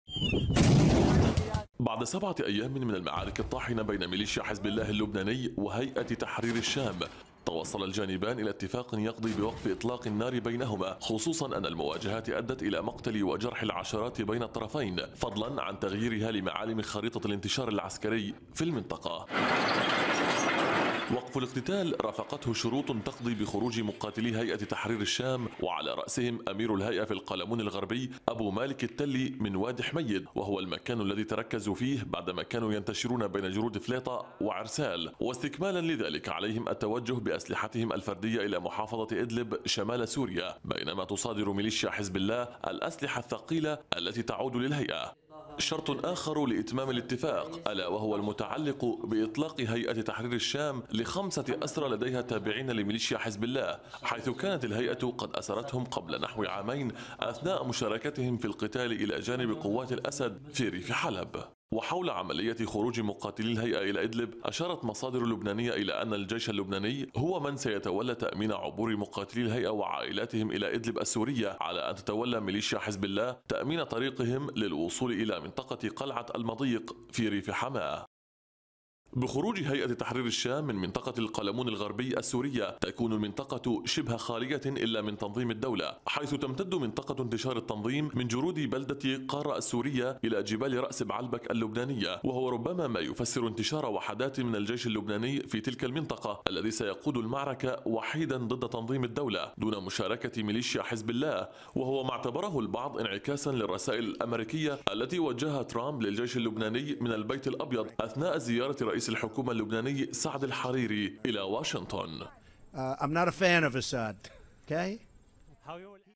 وقف إطلاق النار في جرود عرسال ضمن شروط... هذه أبرزها
أعلنت ميليشيا حزب_الله اللبنانية وقف إطلاق النار في منطقة جرود عرسال القريبة من الحدود السورية اللبنانية، وذلك بعد اتفاق مع هيئة تحرير الشام يقضي بانسحاب مقاتلي الهيئة مع عوائلهم إلى ادلب، مقابل إطلاق سراح خمسة أسرى لحزب الله معتقلين لدى الهيئة منذ نحو عامين.
تقرير: ساري عبد الحق